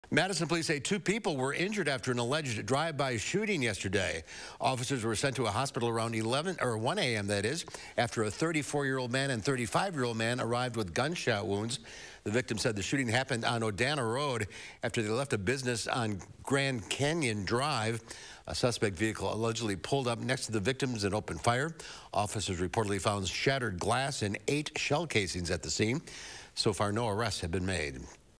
Two injured in alleged drive-by shooting on Odana Road
Two people were injured in an alleged drive-by shooting Sunday, Madison Police said.